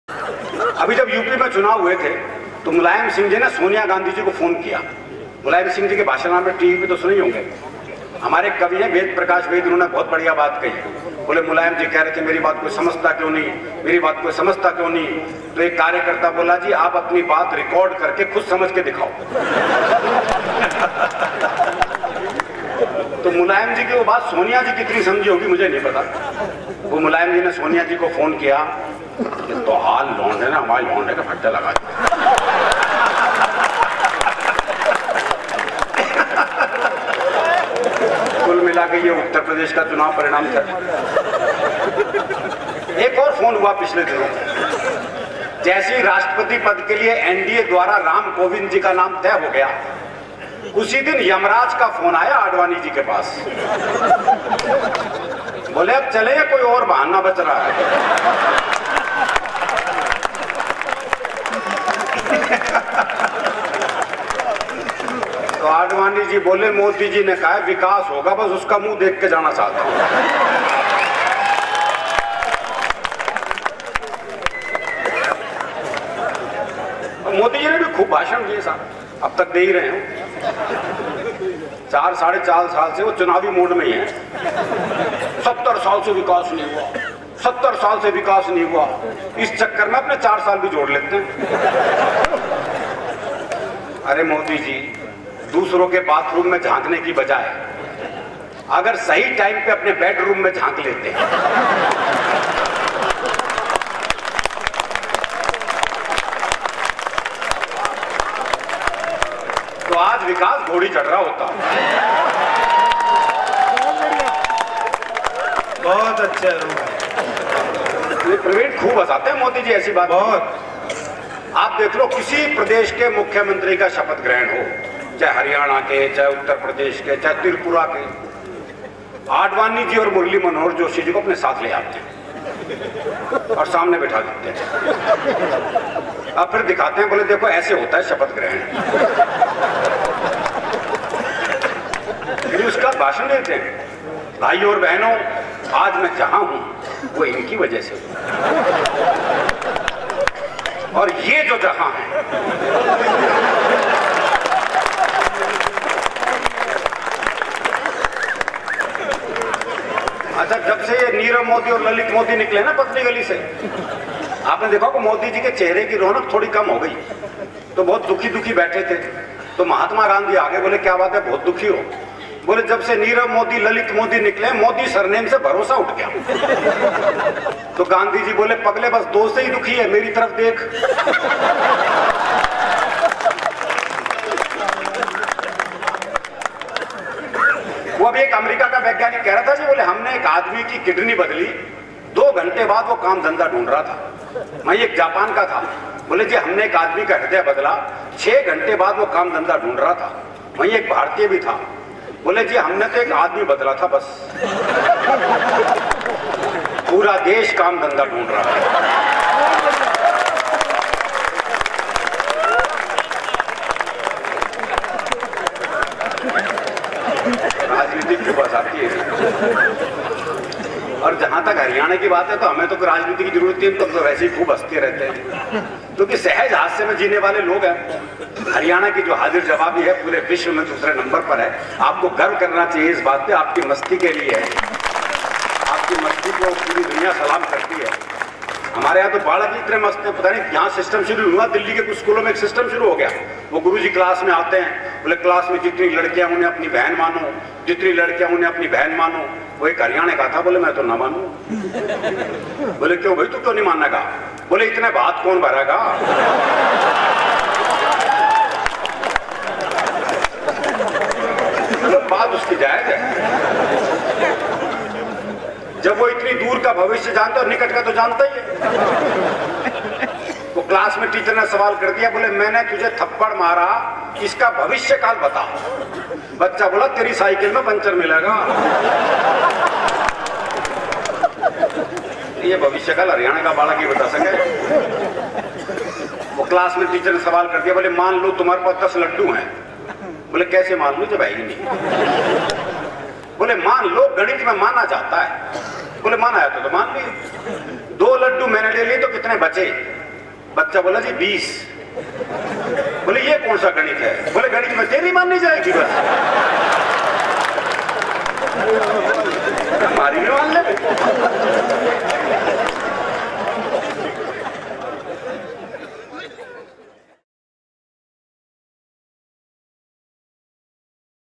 Kamaal Karte Hain ModiJi || Hasya Kavi Arun Gemini
This video contains some hilarious jokes related to Indian politicians. The poet is not intended to hurt any person or party.

This is a copyright video.